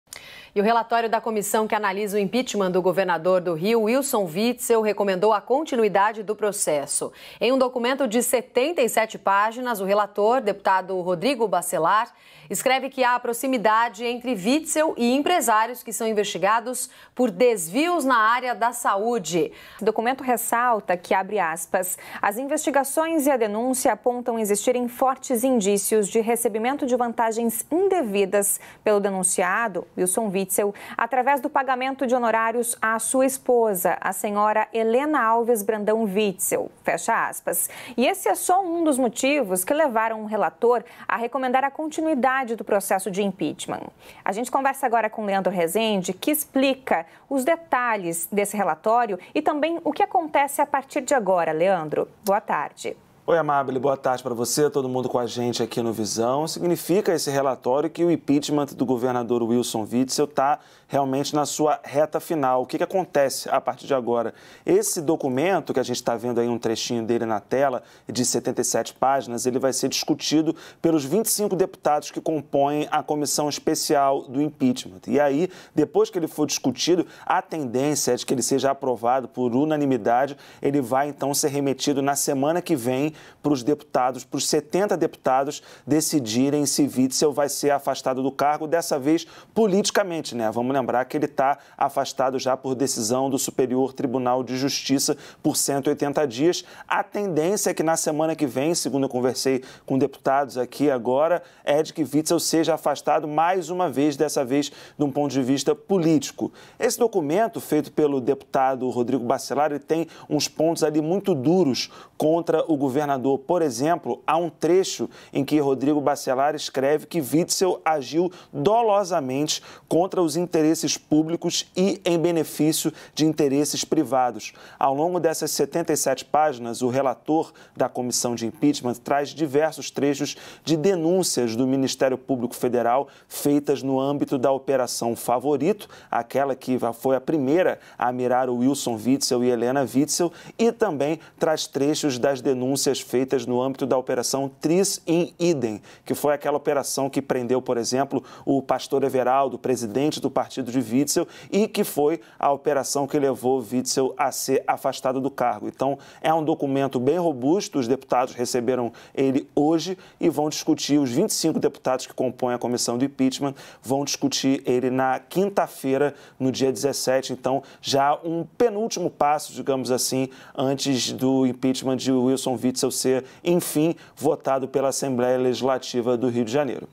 PROCESSO DE IMPEACHMENT DE WITZEL ENTRA NA RETA FINAL E ELE SERÁ CASSADO
Processo de impeachment de Witzel entra na reta final. O relatório não deixa dúvidas: ele será cassado.